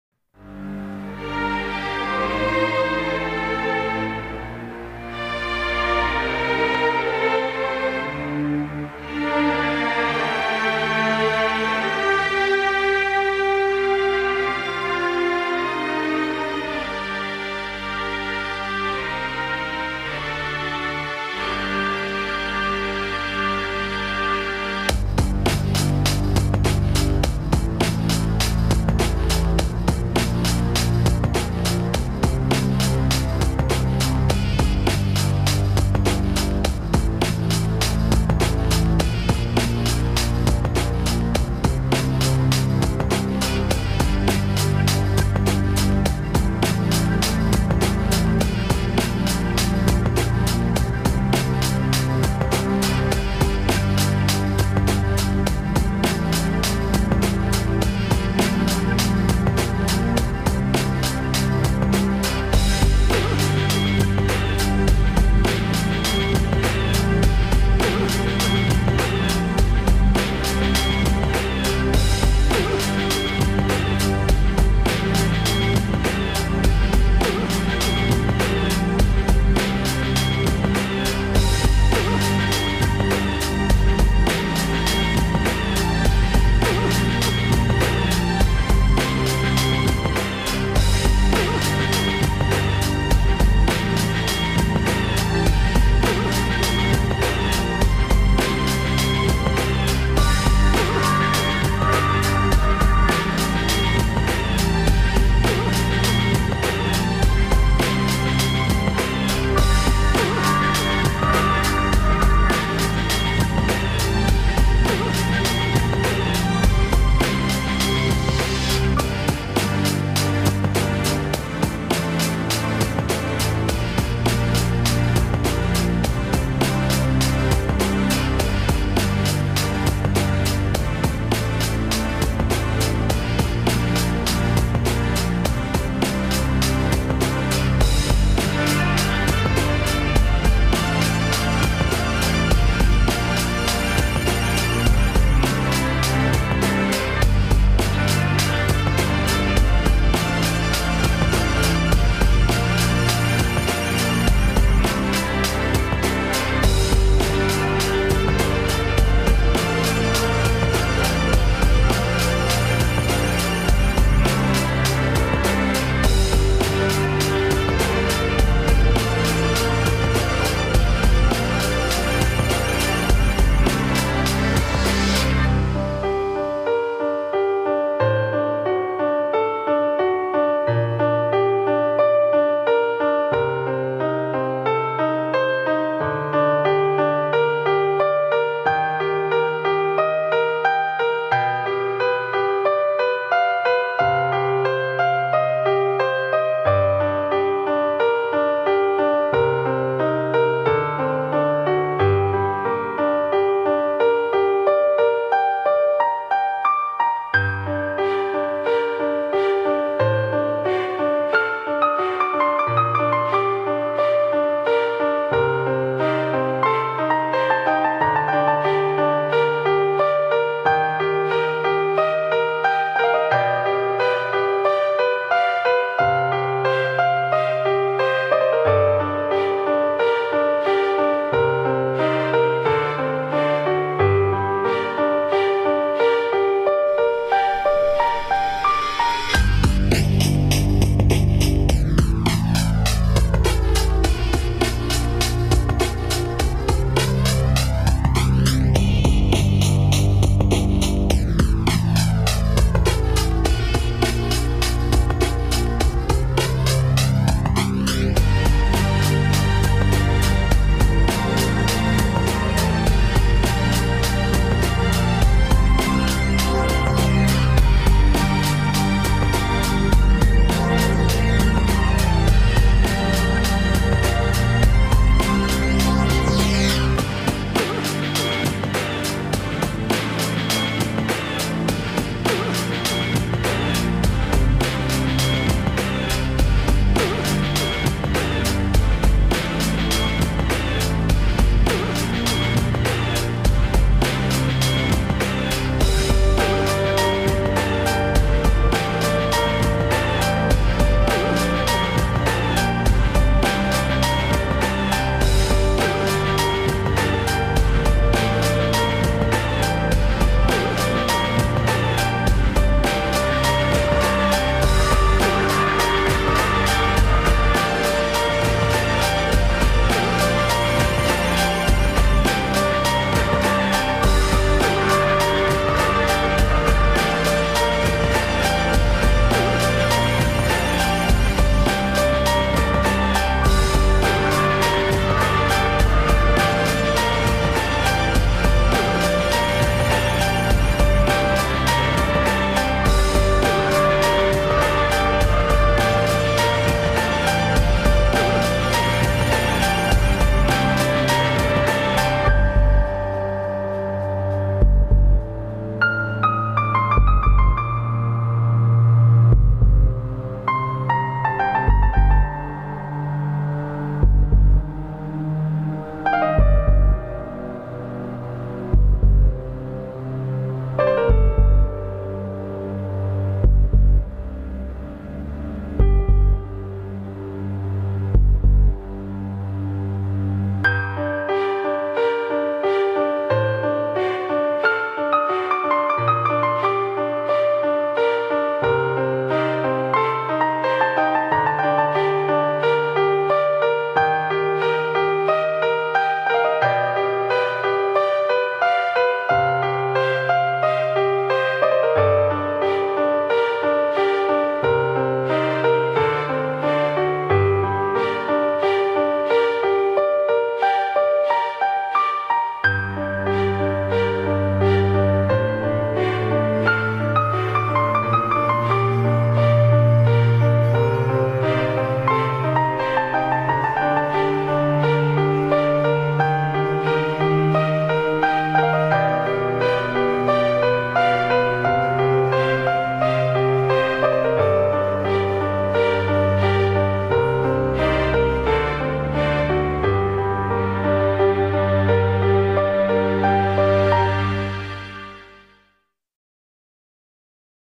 Robotic hand drawing(iron man)
I inspired by seeing pinterest post made by Adi Granov it is digital. soo I did it in traditional with pencil,ink brush and a paper. ✍🦾🙂✌